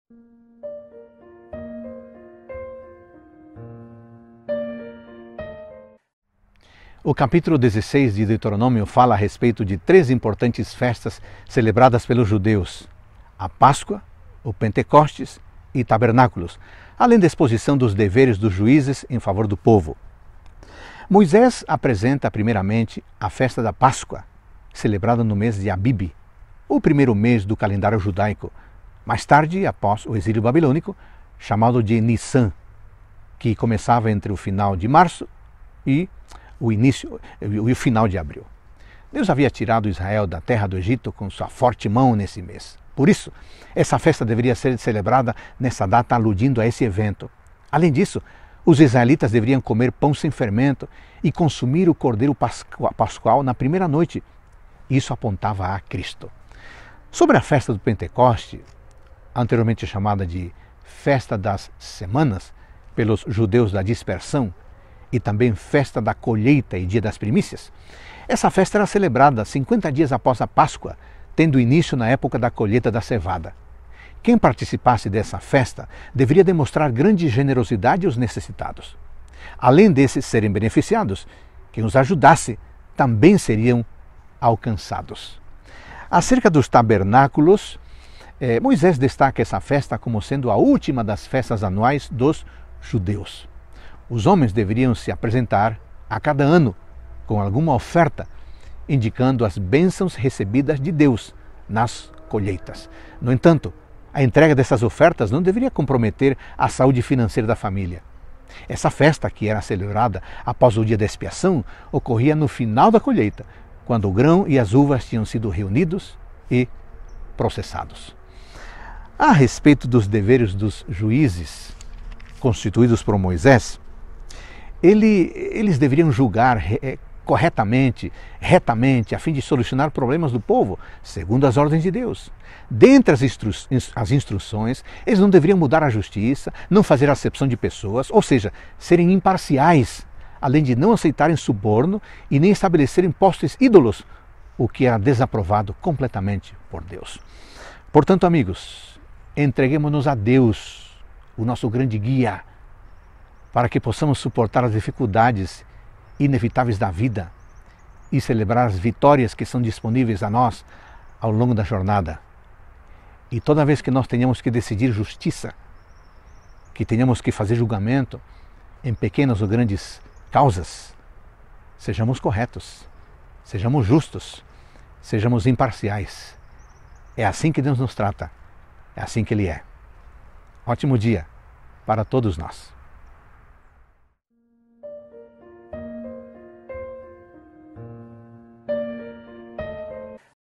Deuteronômio 16 - Reavivados por Sua Palavra
O capítulo 16 do livro de Deuteronômio fala a respeito de três importantes festas celebradas pelos judeus, a saber, a Páscoa, o Pentecostes e os Tabernáculos, além da exposição dos deveres dos juízes em favor do povo.